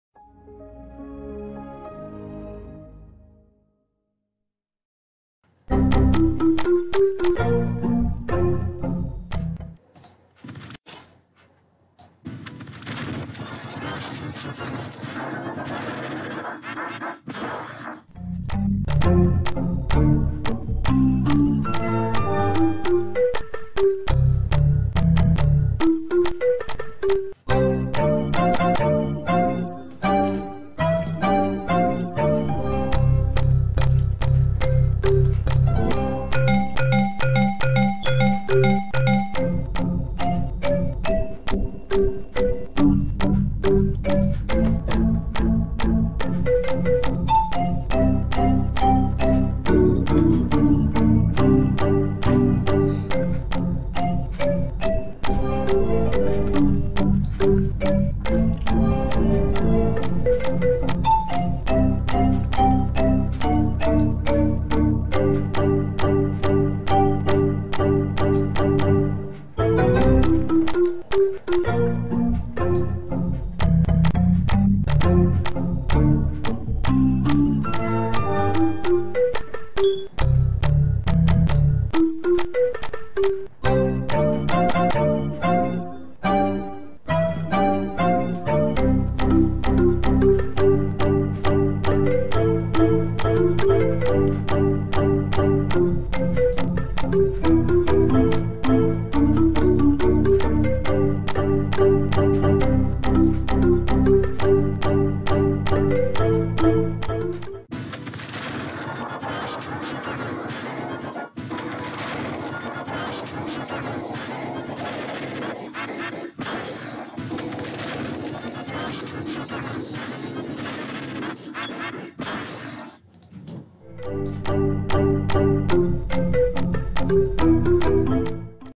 How To Make G-Major 610900 On Vegas Pro
Made on Tuesday, October 12, 2021, 8:22:05 pm to 8:22:55 pm

Let's learn how to make G-Major 610900 made first on Vegas Pro.

How To Make G-Major 610900:

(VideoFX)

1st: Mirror - Reflect Right

2nd: Deform - Divided Effect

Amount:          -0.120
Center Image:     Without A Checkmark
Background Color: (Default)

Squeeze
Left:        1.000
Right:       1.000
Top:         0.000
Bottom:     -1.000

Shear
Horizontal:  1.000
Vertical:   -1.000

Curve
Left:        1.000
Right:       1.000
Top:        -1.000
Bottom:      1.000

3rd: HSL Adjust - Hue 0.054

Add To Hue:  0.054
Saturation:  1.000
Luminance:   1.000

4th: Channel Blend - RGB to BGR

or

3rd: Channel Blend - RGB to BGR

4th: HSL Adjust - Hue 0.946

Add To Hue:  0.946
Saturation:  1.000
Luminance:   1.000

(AudioFX)

elastique
Pro

-24
-12
-8
-5
-3
0
+3
+5
+8
+12
+24

Effect by Carlos Virgílio

(NEW EFFECT) Klasky Csupo in G Major 610900
358 views

Soundtrack

Windows XP
721 views